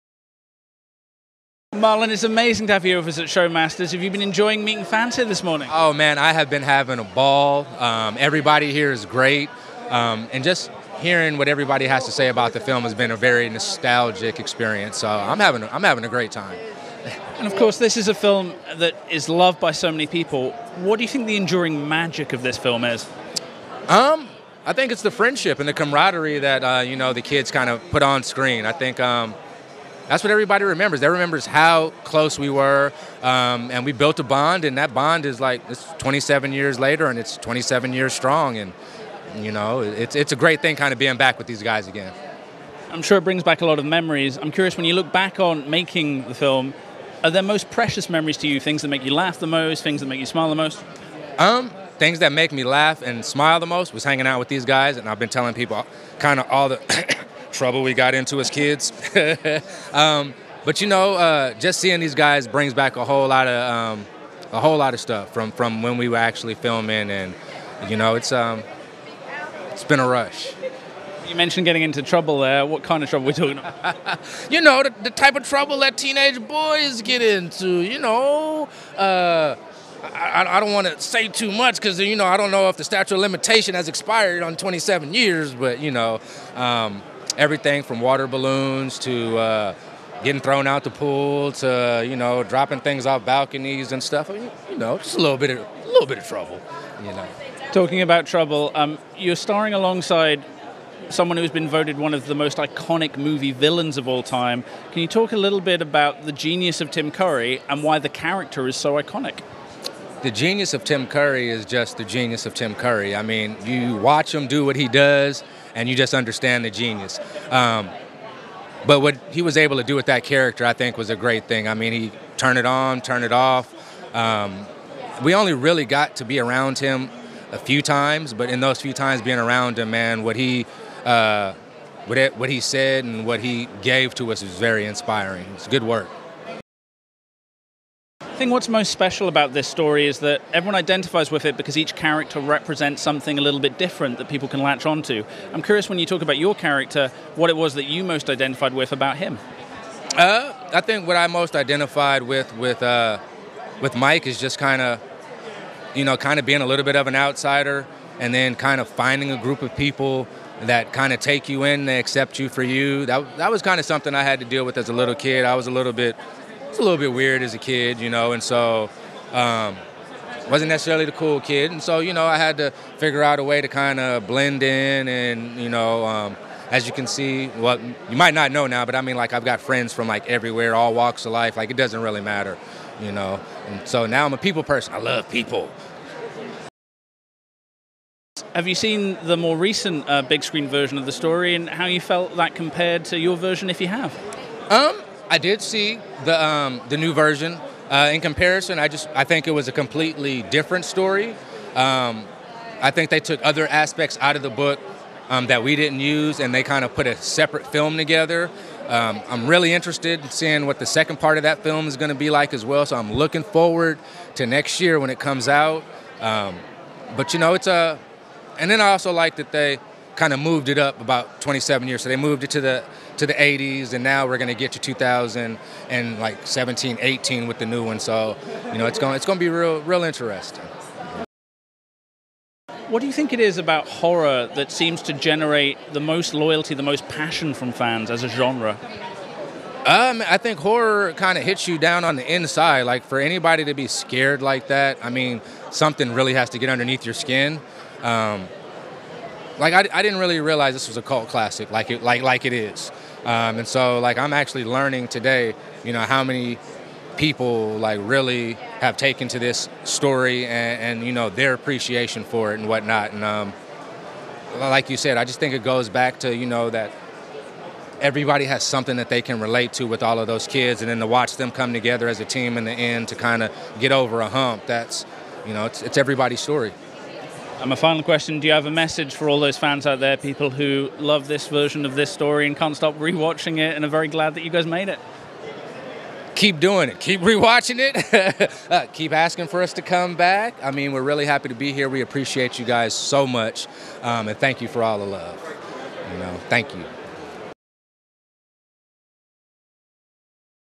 Marlon Taylor Interview Stephen King's IT (1990)
Red Carpet News TV talks to VIP guest at the London Film and Comic Con Spring 2019. Interviews include Iain Glen, David Warner, Eddie Marsen, Mark Jackson, Lawrence Gillard Jr, Karyn Parsons, Christopher Fairbank and Stephen King’s IT 1990 stars Adam Faraizl, Ben Heller, Brandon Crane, Marlon Taylor & Emily Perkins.